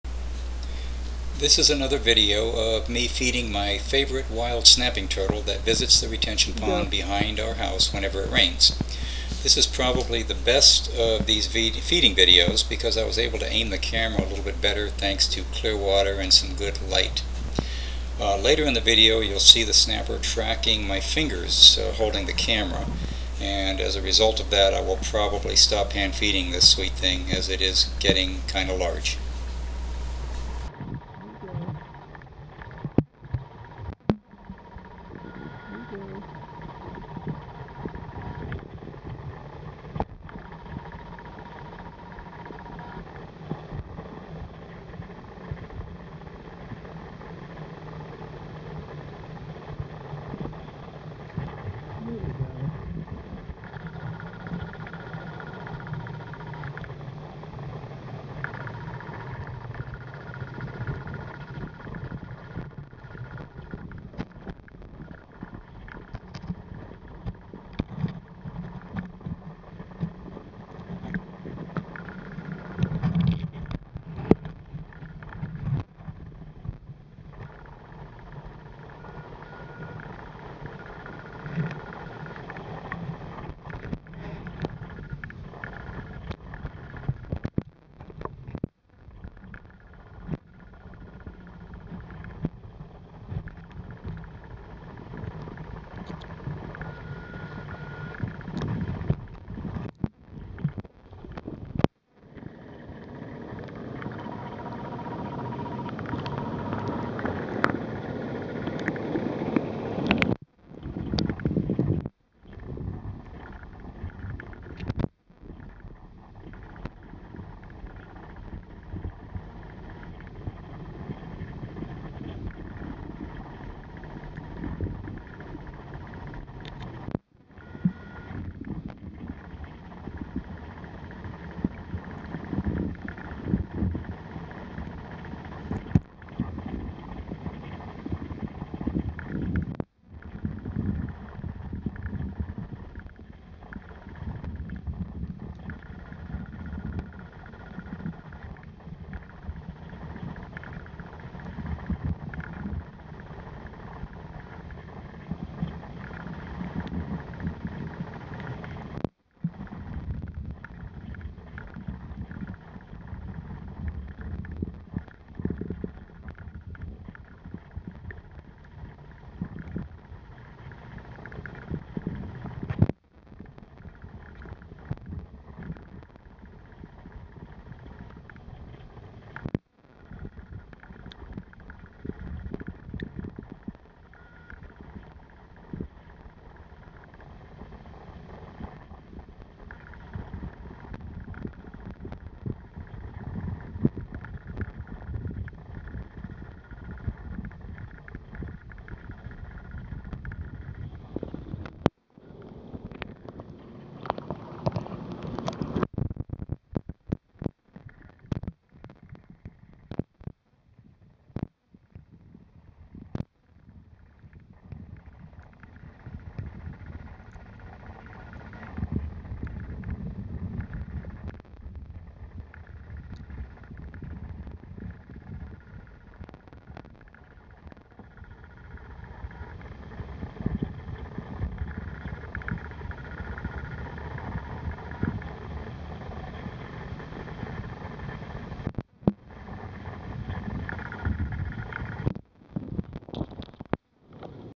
Feeding A Wild Snapping Turtle - August 15, 2011
This is one of several videos I have made feeding a wild snapping turtle that frequents the retention pond behind our home.  I have been photographing and feeding this turtle for a few years now.  This video is the best because the water was clear and the lighting was good, was able to keep the camera pointing at the turtle's head.  Feeding a snappig turtle by hand is not really a good idea and the big guy has now started tracking my fingers holding the camera.  You can see that at one point in the video.  I will probably not hand feed this big sweetie anymore.  :)